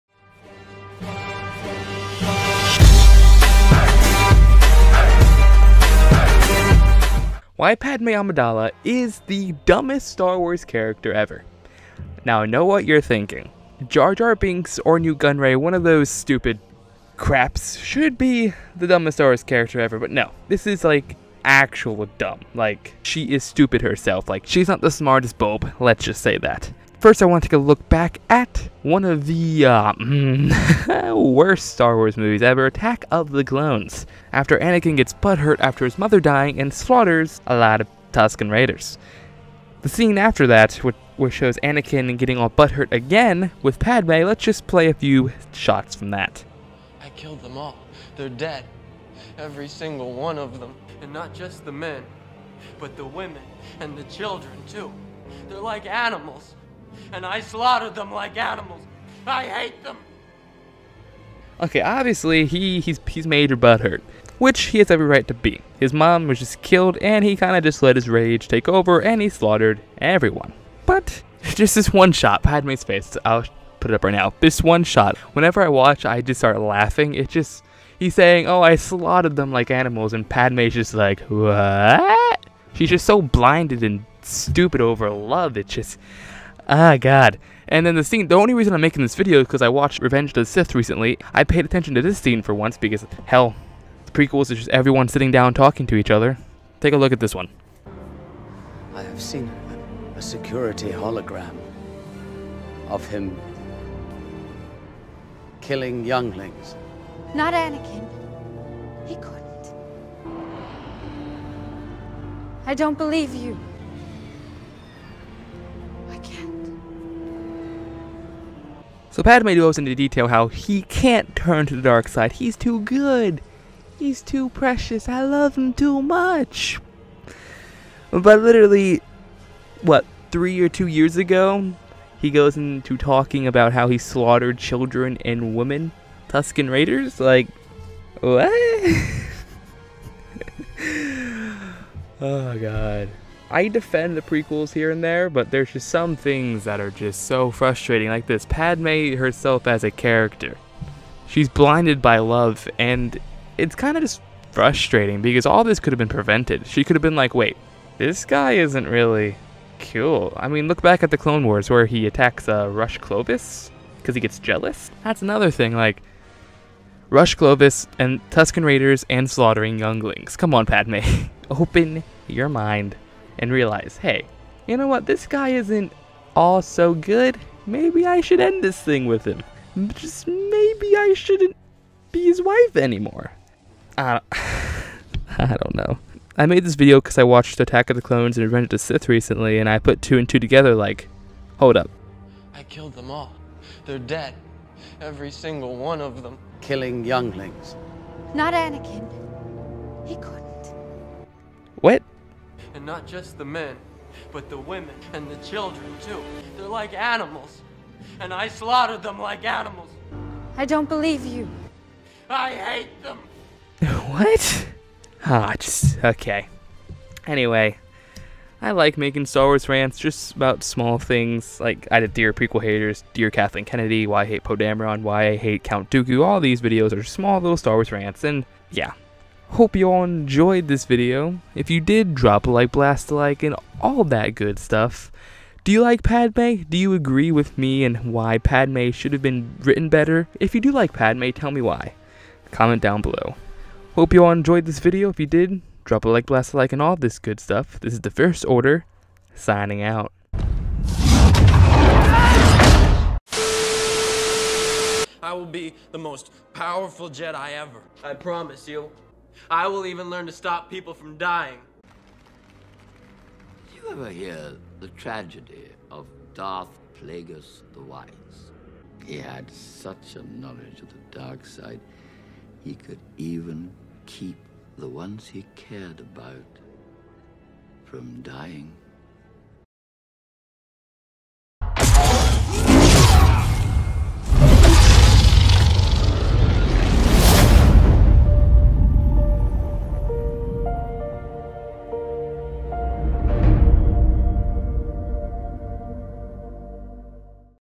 Why I HATE Padme Amidala!
The why I hate (insert dumb/useless/clueless/poorly written/ annoying) character here returns! And this time I dive into one of the most blind characters in all of Star Wars... No not Chirrut.. Smh. Padme Amidala!